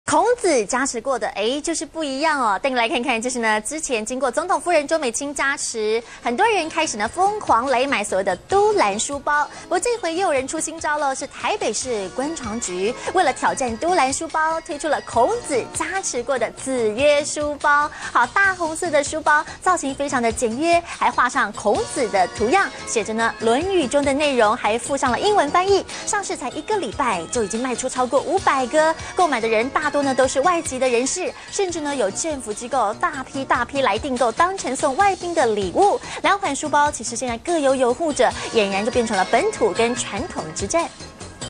一週賣逾500個「子曰書包」夯受外國人青睞
中天新聞》經總統夫人周美青加持，國人瘋狂購買「都蘭書包」，台北市觀傳局為挑戰都蘭書包，推出孔子加持的「子曰書包」，上市僅一周已賣出逾500個，購買者大多是外籍人士，甚至有政府機構大批訂購，當成送外賓的禮物，2款書包各有擁護者，儼然成為本土與傳統之戰。